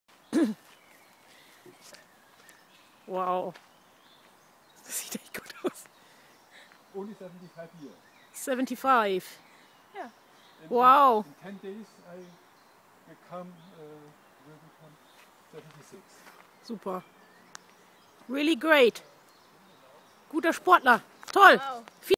Personal Training Bremen Fitnessmotivation: Gesund alt werden
Personal Fitness Training für Senioren ist zum Erhalt der Fitness im Alter gut geeignet. Dieser 75 jährigen Turner im Bremer Bürgerpark ist "fit, wie ein Turnschuh" durch sein regelmässiges Fitness Training. Er hat eine hohe Sportmotivation und trainiert eigenständig.
Es ist besonders wichtig die Muskulatur zu erhalten bzw. aufzubauen. Sie unterstützt die gute Haltung, die Stoffwechselaktivität und hilft besonders dabei Erkrankungen des Bewegungsapparates und Verletzungen zu verhindern. Das Motto ist "gesund & jung alt werden" - das bedeutet Wohlbefinden und Lebensqualität im Alter.